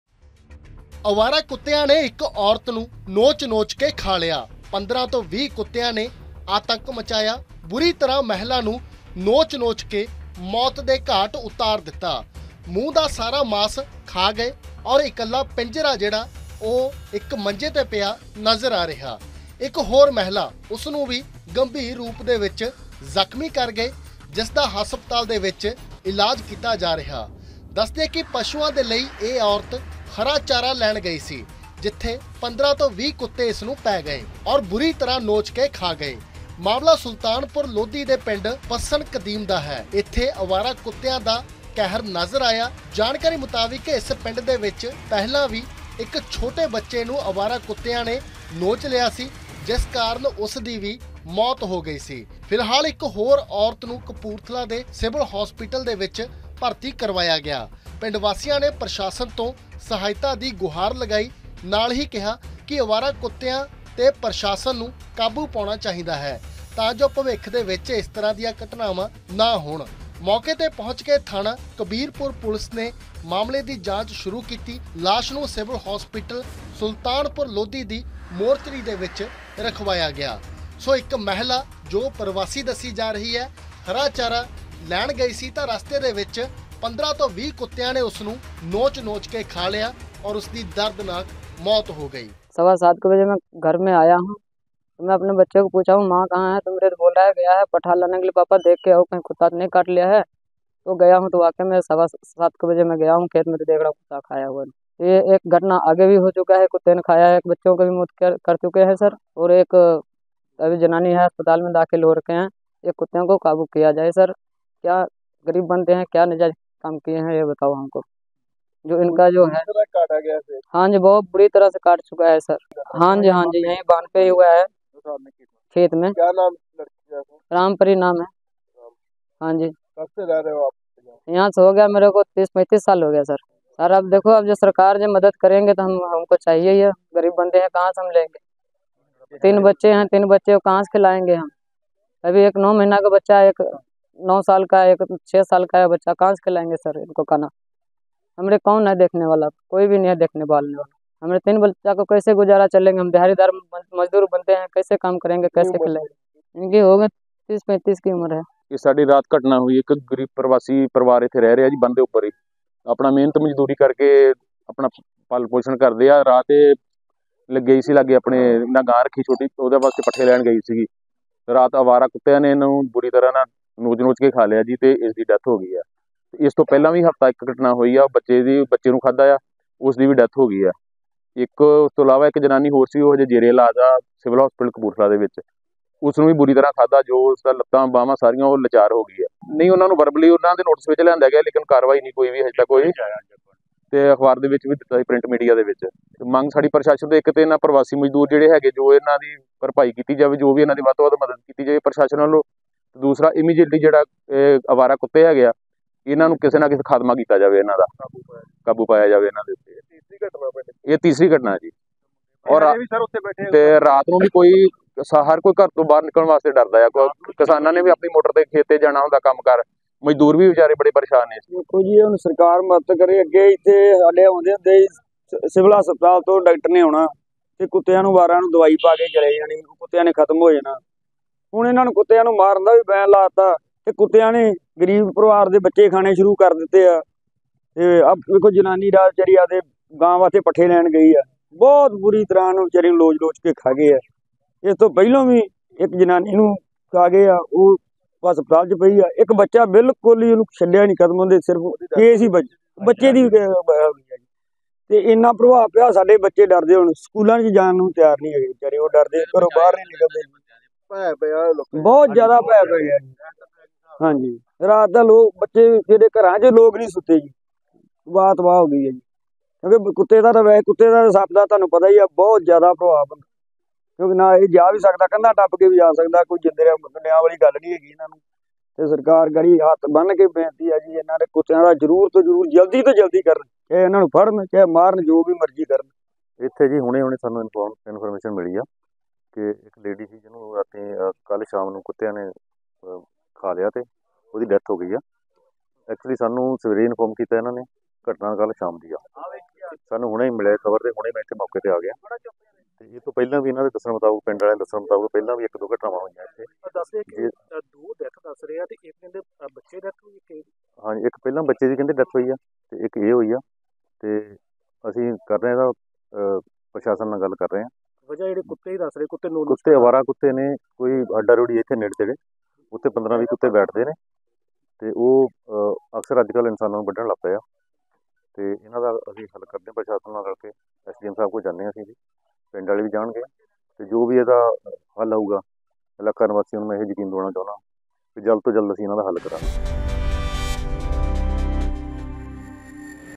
Kapurthala News | ਚਾਰਾ ਲੈਣ ਗਈ ਔਰਤ ਨੂੰ ਨੋਚ ਖਾ ਗਏ ਕੁੱਤੇ, 1 ਹੋਰ ਨੂੰ ਕੀਤਾ ਜ਼ਖਮੀ | Dog Attack
ਕਪੂਰਥਲਾ ਦੇ ਸੁਲਤਾਨਪੁਰ ਲੋਧੀ ਦੇ ਪਿੰਡ ਪੱਸਣ ਕਦੀਮ ਵਿਖੇ ਇੱਕ ਬਹੁਤ ਹੀ ਦਰਦਨਾਕ ਘਟਨਾ ਵਾਪਰੀ ਹੈ, ਜਿੱਥੇ ਕਿ ਅਵਾਰਾ ਕੁੱਤਿਆ ਦੀ ਦਹਿਸ਼ਤ ਇਨੀ ਵੱਧ ਗਈ ਅਵਾਰਾ ਕੁੱਤਿਆਂ ਵੱਲੋਂ ਇੱਕ ਪ੍ਰਵਾਸੀ ਔਰਤ ਨੂੰ ਨੋਚ ਨੋਚ ਕੇ ਮੌਤ ਦੇ ਘਾਟ ਉਤਾਰ ਦਿੱਤਾ ਹੈ। ਜਦ ਕਿ ਇੱਕ ਮਹਿਲਾ ਸਿਵਿਲ ਹਸਪਤਾਲ ਕਪੂਰਥਲਾ ਵਿਖੇ ਇਲਾਜ ਅਧੀਨ ਹੈ।